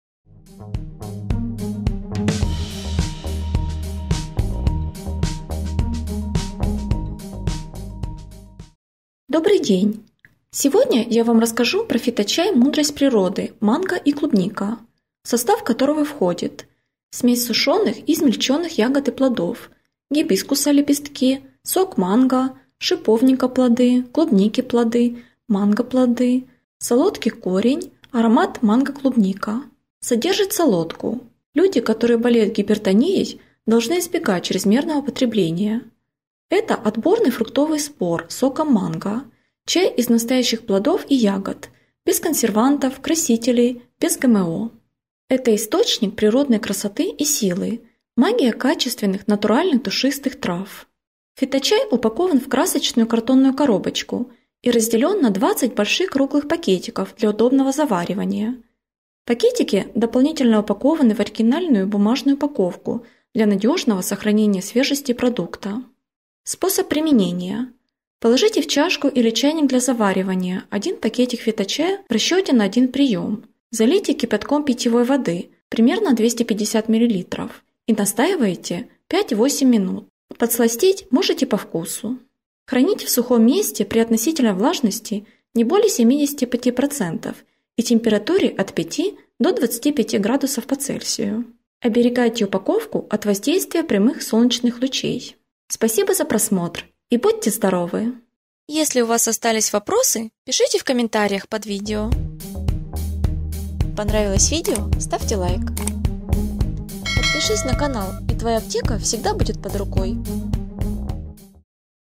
Фиточай Мудрость природы Манго и клубника - показания, описание, отзывы
МАНГО І ПОЛУНИЦЯ
Відбірний фруктовий збір з соком манго. Чай зі справжніх плодів і ягід. Без жодних консервантів, барвників, без ГМО. Джерело природної краси та сили. Природньо багатий вітамінами. Натуральна якість. Барвиста картонна коробочка. 20 великих круглих пакетиків чаю для найкращого заварювання. Пакетики додатково запаковані в оригінальну паперову скриньку для надійного збереження свіжості продукту.

Склад: суміш сушених і подрібнених ягід та плодів: гібіскусу пелюстки,сік манго, шипшини плоди, полуниці плоди, манго плоди, солодка корінь, аромат манго-полуниця

❤️ Внимание!